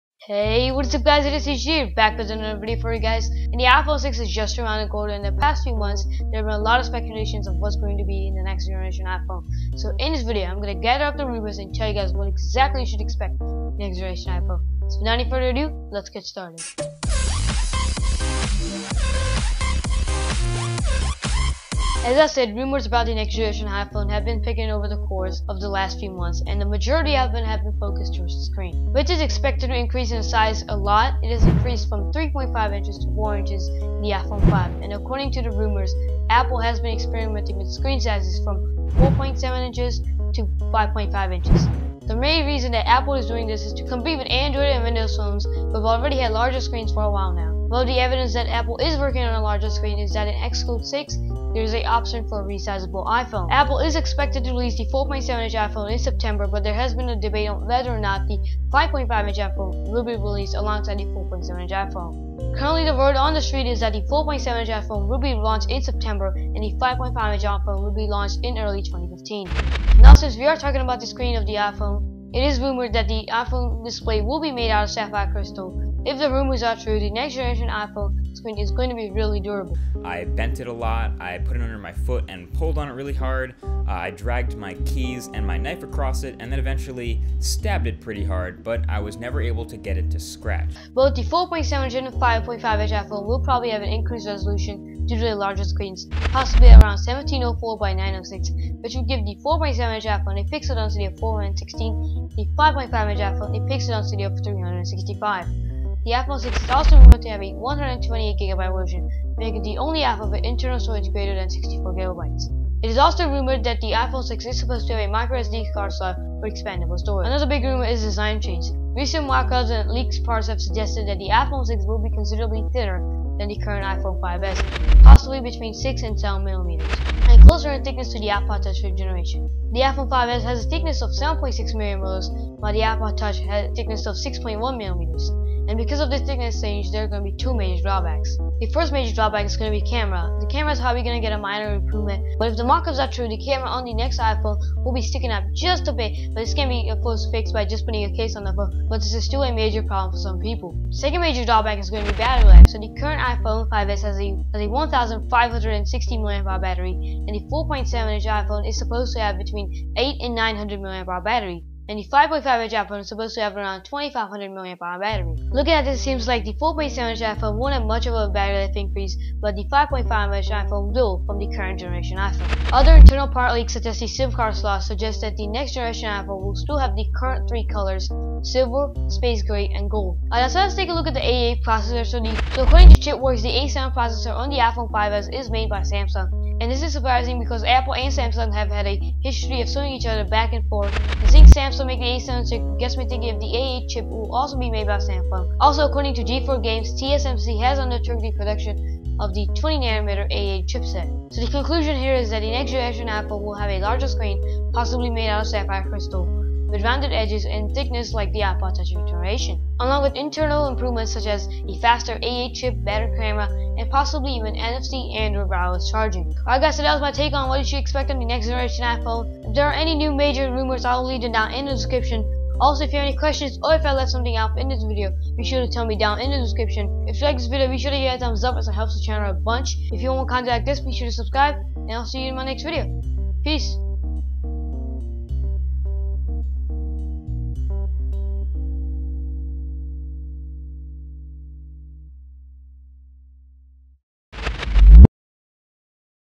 iPhone 6 | What to Expect? | Rumor Roundup
The next generation iPhone is just around the corner and in this video I go over exactly what you should expect from the next iPhone!
UPDATE August 7th 2014: September 9th event is confirmed.

UPDATE August 8th 2014:
- Next Generation iPhone release date to be September 6th 2014; Acoording to mutiple sources such as Re/code sat that it is scheduled . Noting has been said by Apple.
- The supply chain checks report that screen sizes won't be the only thing that differentiate the iPhone; The 5.5" iPhone may include a different touch module from the 4.7" iPhone. The analyst also that the processes will also be larger in size!

Other Sources are listed in the video.

I DO NOT OWN MOST IMAGES AND VIDEOS CLIPS SHOWN IN THIS VIDEO.

DDSHD